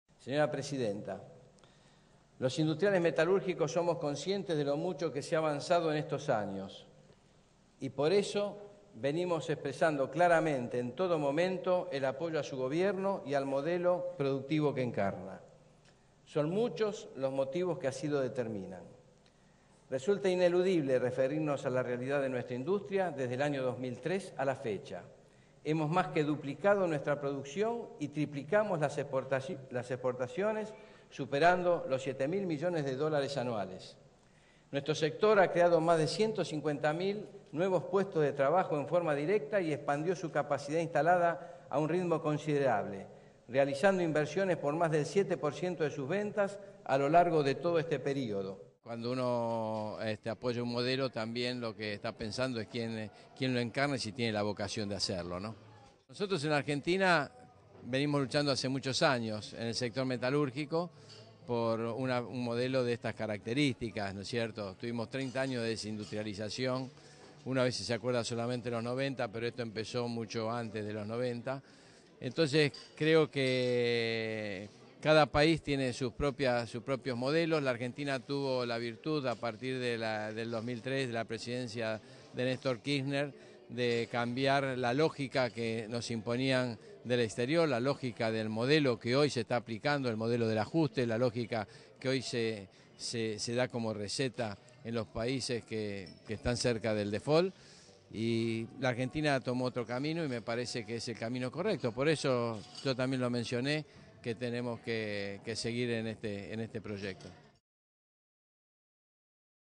"Venimos luchando hace muchos años por un modelo de estas características".
El presidente de la Asociación de Industriales Metalúrgicos de la República Argentina (ADIMRA), Juan Carlos Lacurain, advirtió que desde el sector se viene "luchando desde hace muchos años por un modelo" como el que se aplica en la Argentina desde el año 2003.
"Señora Presidenta: los industriales metalúrgicos venimos expresando el apoyo a su gobierno y al modelo que encarna", expresó Lascurain, durante el discurso de cierre del Congreso Metalúrgico 2011, que brindó frente a Cristina Fernández de Kirchner.
Luego de la ceremonia, Lascurain reiteró estos conceptos en declaraciones a Télam y comparó  la situación argentina con la que atraviesan los países centrales.
"Argentina tomó otro camino y es el camino correcto", sentención el titular de ADIMRA.